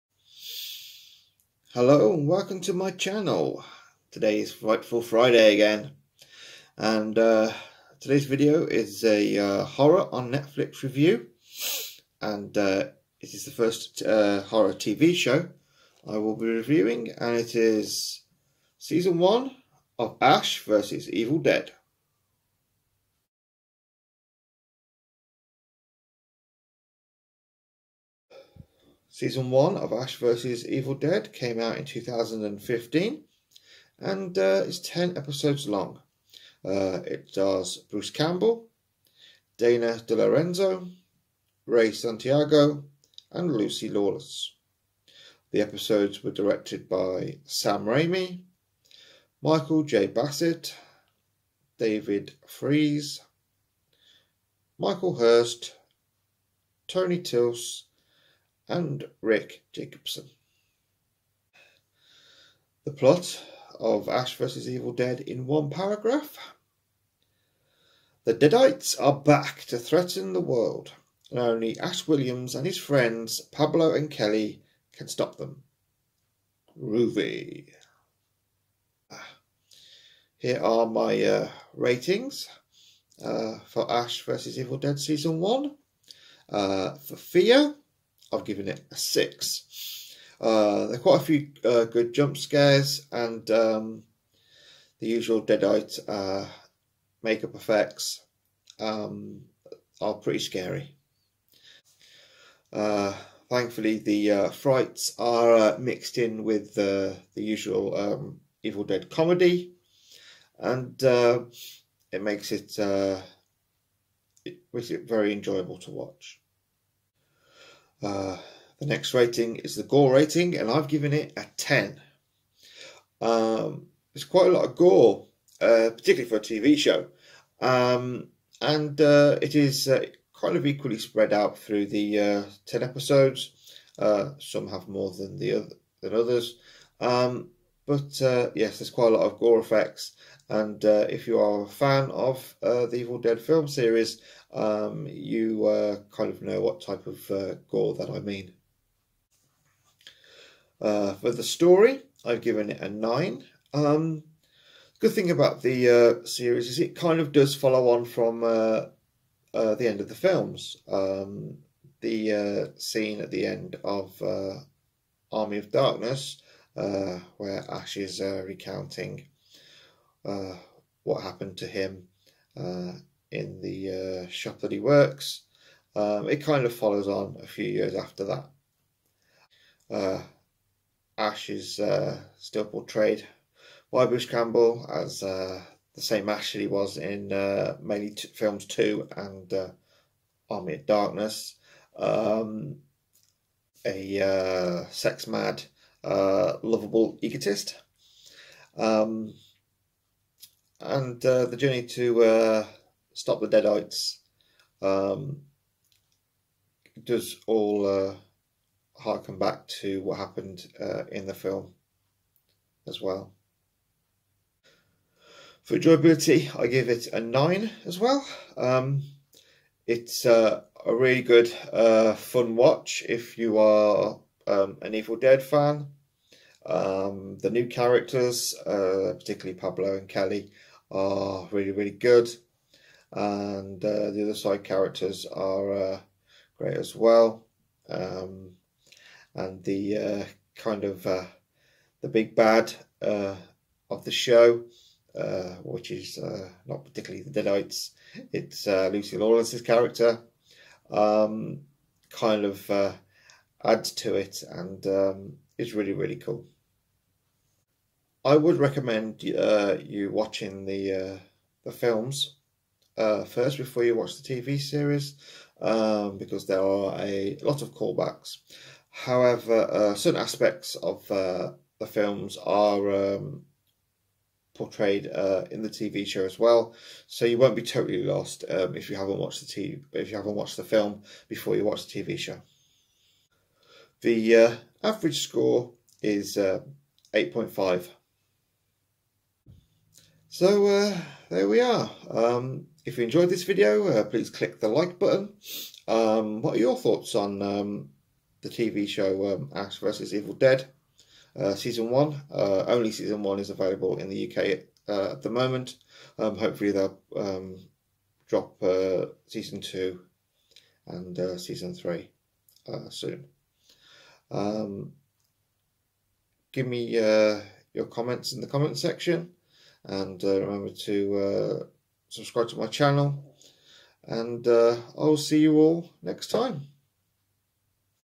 FrightfulFriday Horror on Netflix UK Ash vs Evil Dead Season 1 Review
My review of the horror comedy TV show, Ash vs Evil Dead Season 1.

Grade System on Ratings Average Score:
F   (Under 3)